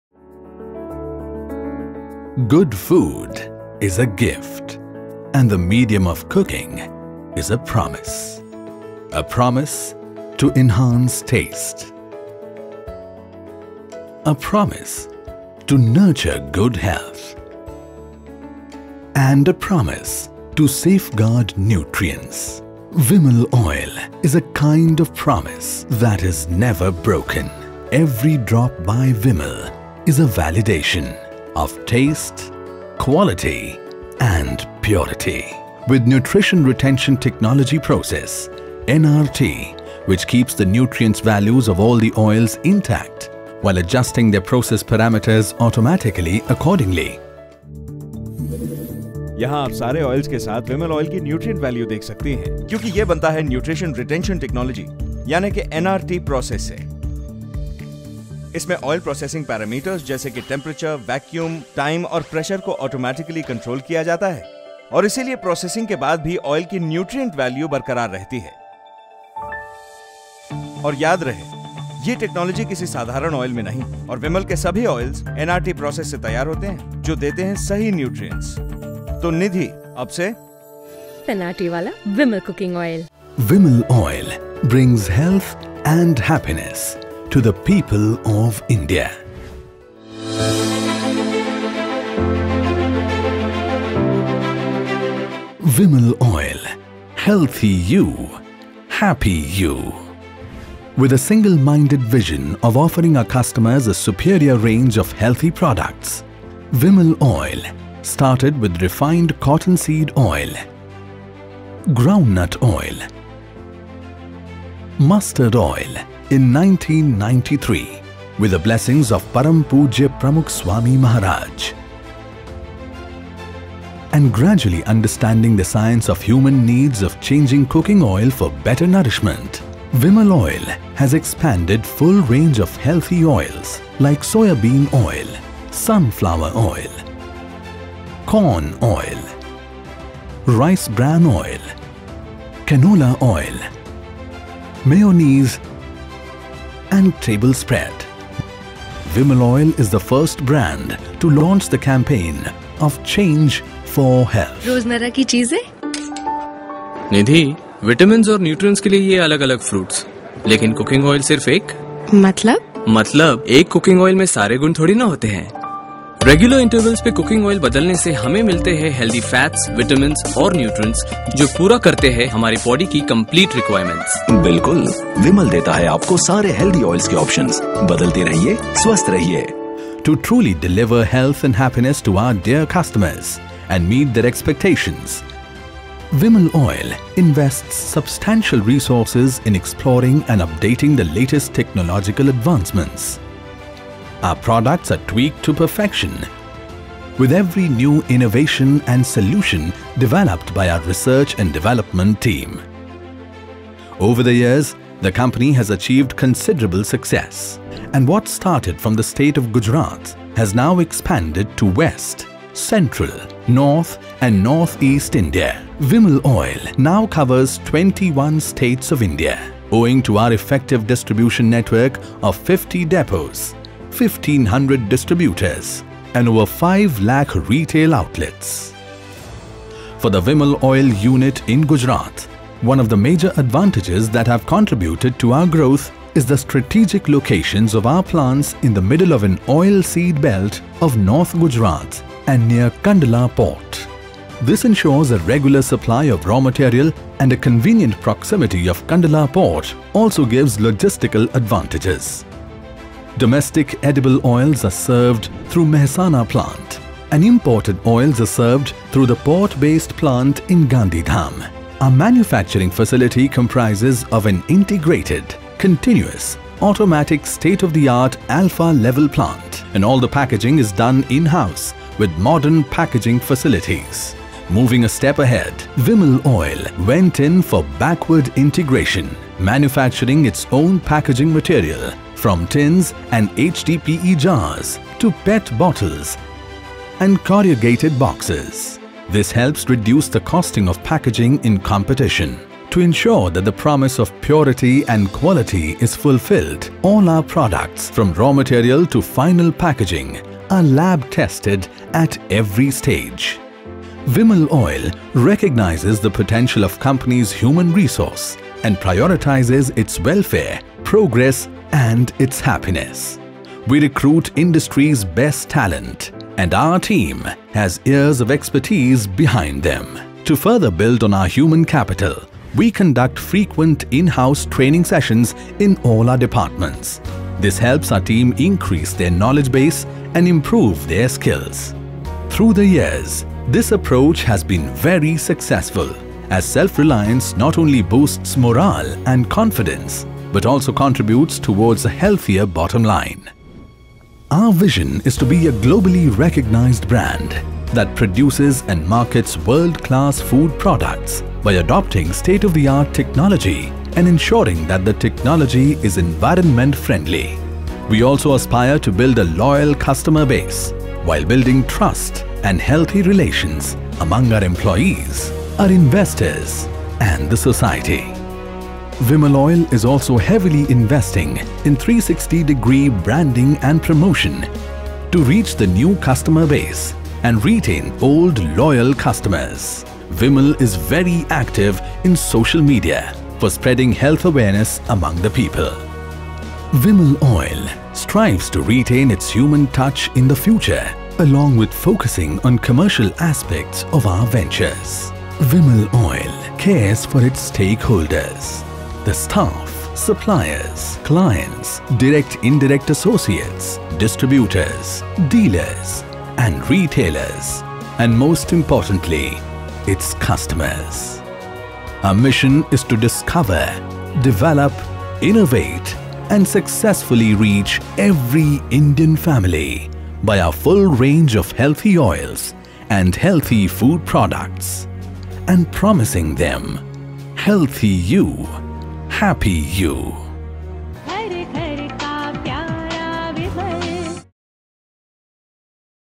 Vimal Oil I Corporate Film I Vimal Oils & Foods I Decode Mediacom
"Journey through the heart of nourishment with Vimal Oil's captivating corporate film, meticulously crafted by the creative geniuses at Decode Mediacom. Witness the dedication and passion that Vimal Oils & Foods pour into each product, as this film unravels the story of purity, quality, and trust. From the farm to your kitchen, experience the essence of Vimal's commitment to excellence, presented with a touch of cinematic brilliance by the Decode Mediacom team. Discover the legacy of Vimal Oil, where tradition meets innovation, and taste the difference that has made them a household name."